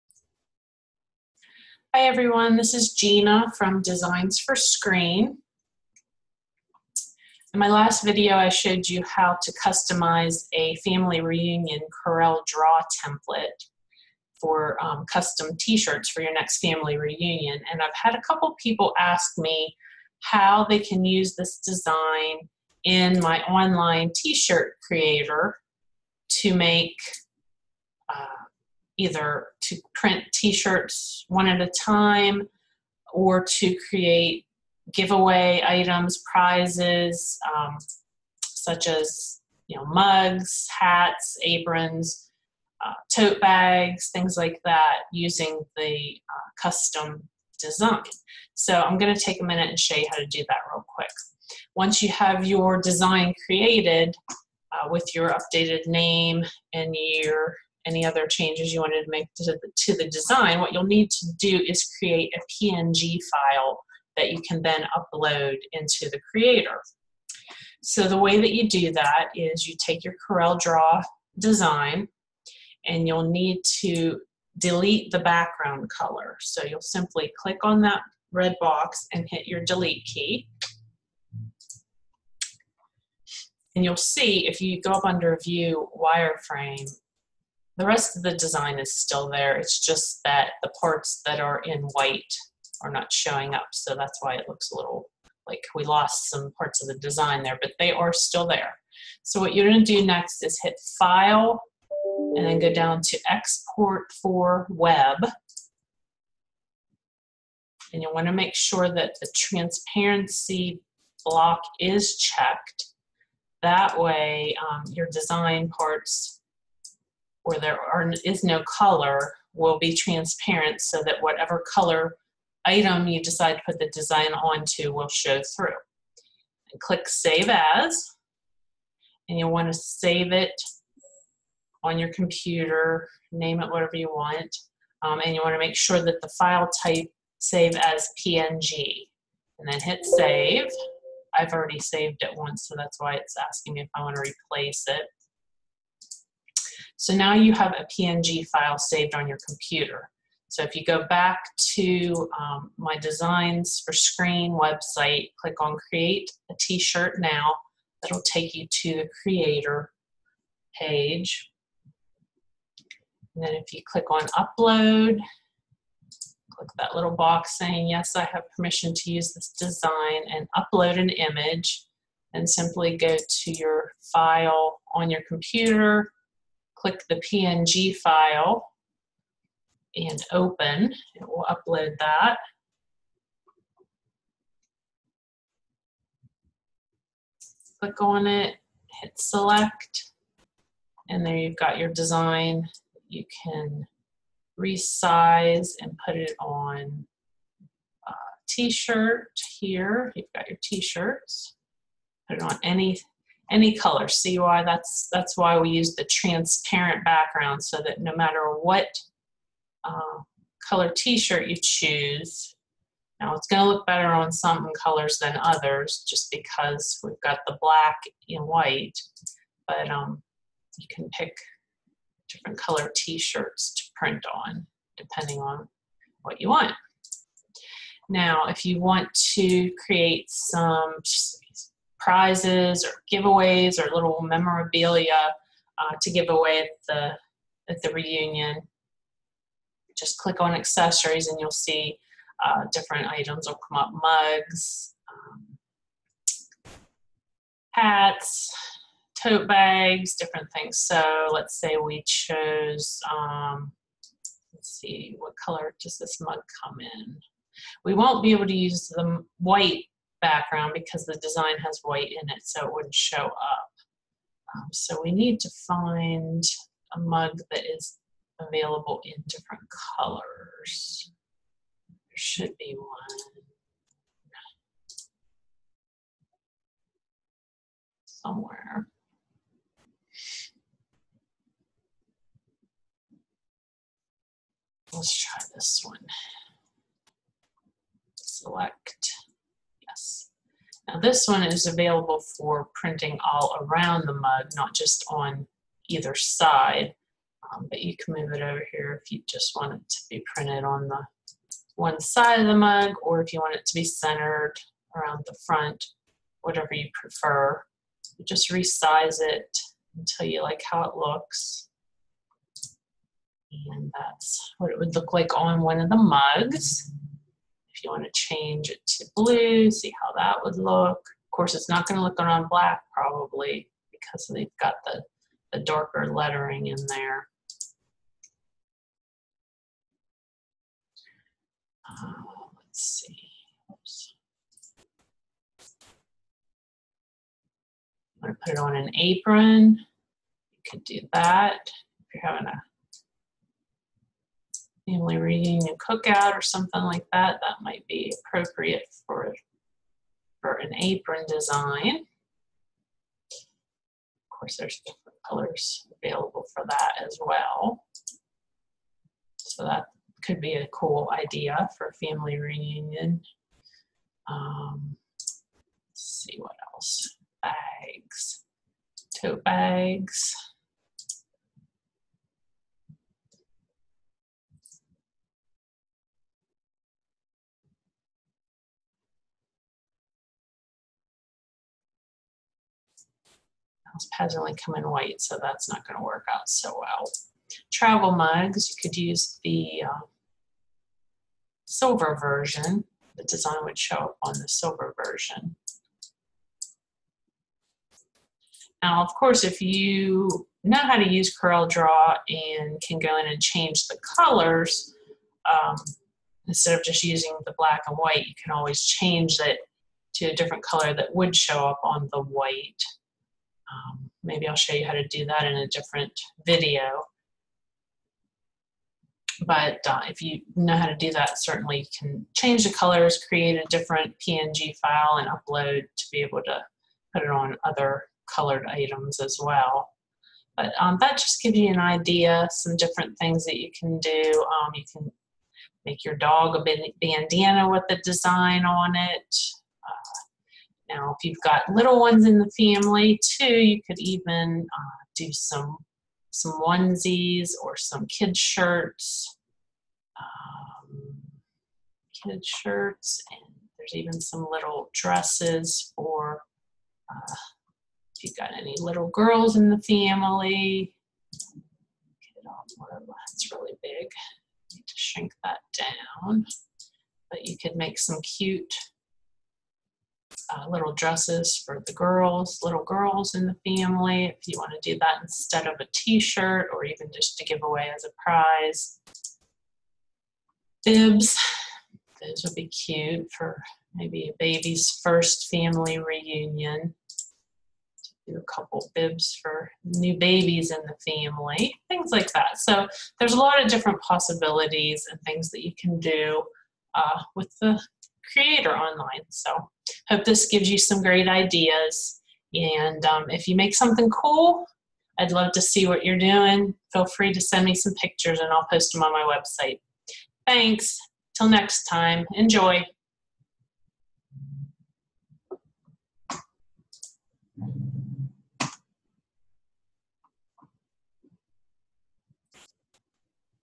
CorelDraw T Shirt Tutorial - How to Create Transparent PNG Files - Family Reunion T Shirt Designs
This short video tutorial shows how to easily take your customized CorelDraw template design and create a transparent PNG file to upload into the online t-shirt designer to create custom t-shirts, mugs, aprons, bibs, and other memorabilia to give away as prizes at your family reunion.